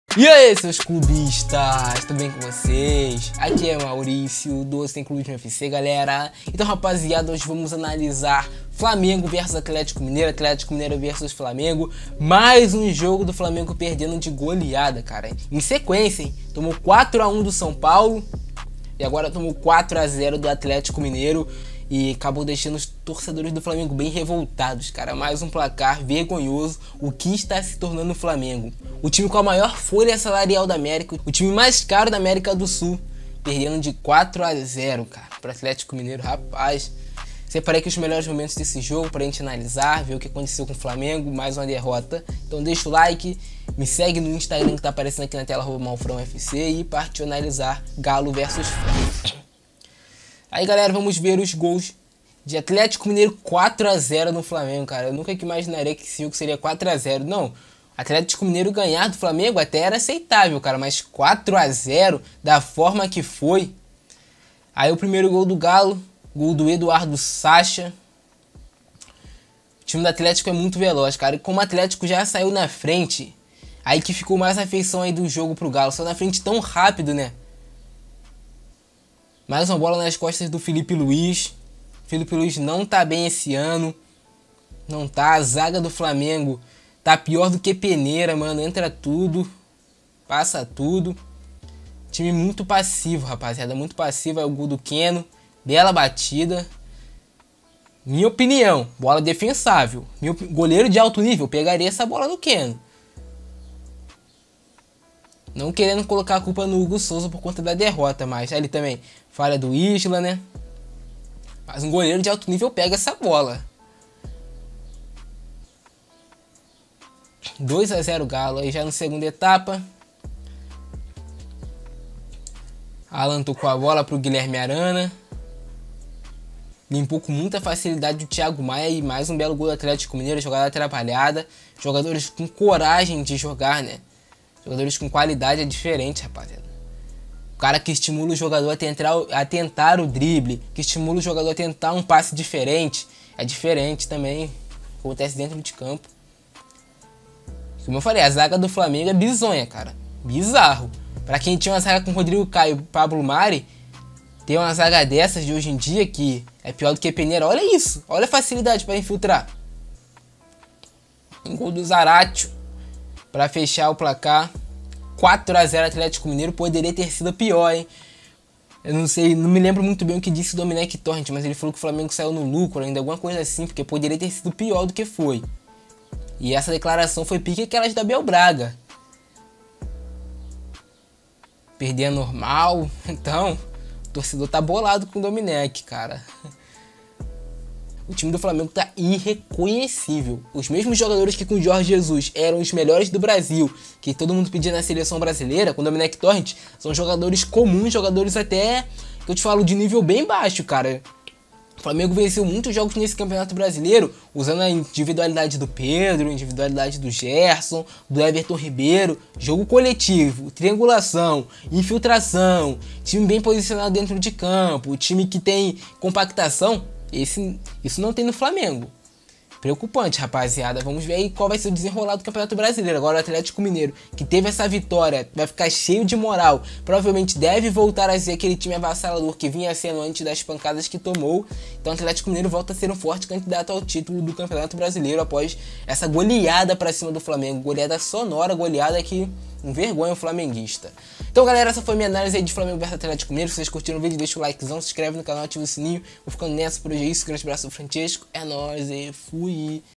ATLETICO MG HUMILHA O FLAMENGO E DERRUBA DOMENEC TORRENT
Então galera hoje vamos analisar flamengo x atletico mg, jogo que o atletico mg humilhou o flamengo e derrubou o treinador domenec torrent, o galo aplicou uma goleada de 4x0 pra cima do flamengo, goleada sonora e humilahnte, vamos ver os gols e analisar flamengo x atletico mg.

LOJA